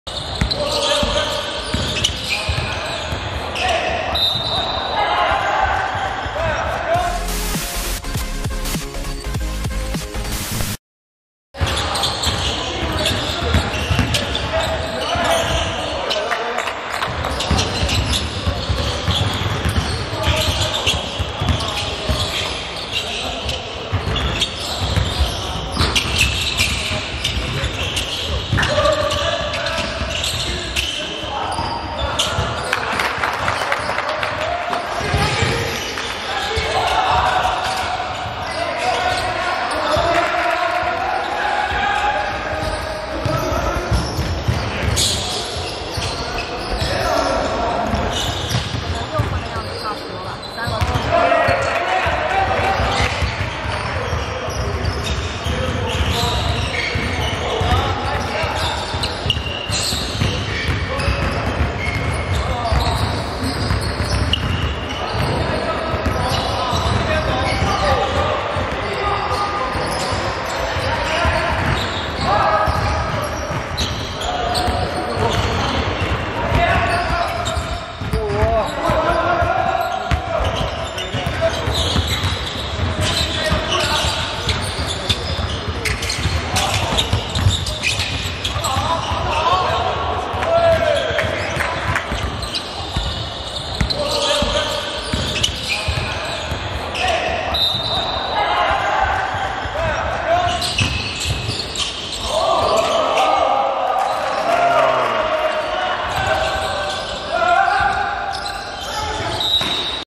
林秉圣&吴永盛：CBA选秀训练营第二天全集锦 | 2019CBA选秀
两人发挥在训练营中都比较出色
有机会在今年的选秀中被选中